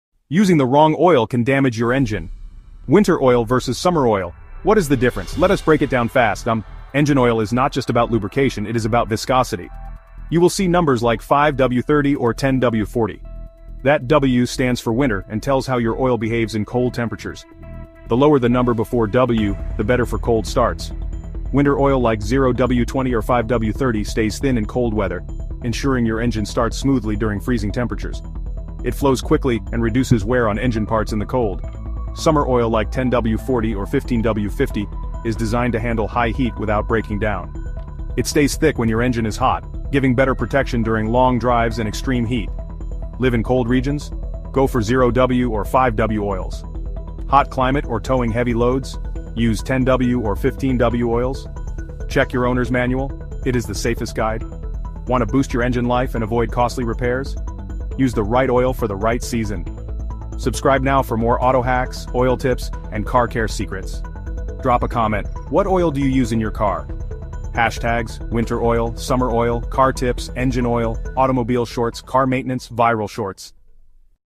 "Winter Oil vs Summer Oil – Which to Use?"
WELCOME TO
               "AUTOMOBILE GARAGE HUB"
❄️🔥 Winter Oil vs Summer Oil – What’s the Difference?
Using the wrong engine oil in the wrong season can DAMAGE your engine! In just 2 minutes, this Short explains how engine oil viscosity works, and which oil is best for hot vs cold weather conditions. 🛢️

✔️ Learn the meaning of 5W-30, 10W-40, and more
✔️ Understand when to use Winter Oil (like 0W-20)
✔️ Find out which Summer Oil protects best in heat
✔️ Improve cold starts and engine life with the right oil!

📘 Always check your owner's manual before choosing your oil.
🔧 Keep your engine running smoothly all year round!

👉 Drop a comment: What oil do YOU use in your car?